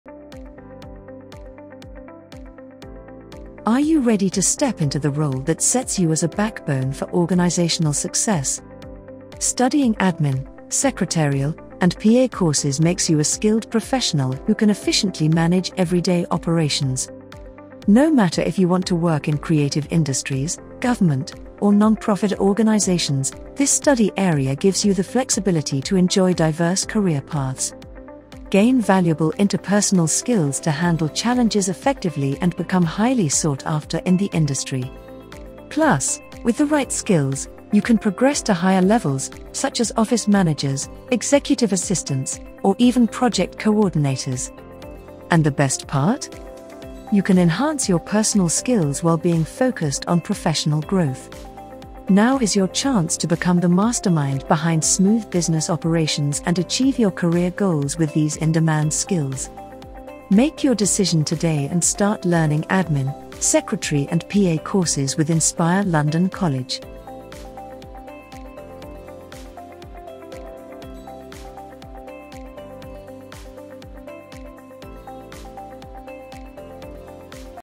Why Study Admin, Secretarial, & PA
Welcome To Inspire London College!
Study Admin, Secretarial, & PA courses equips you with the essential skills to efficiently manage daily business operations, making you a vital asset in any organisation. These courses offer versatile career opportunities accross various industries, from corporate to government and non profit sectors. You'll gain interpersonal skills and organisational expertise, enabling you to handle workplace challenges and advance to roles like office manager, executive assistant, or project coordinator. Enhance both personal and professional growth. While setting the stage for career success. Start your journey today with Inspire London College and master the skills that keep businesses running smoothly.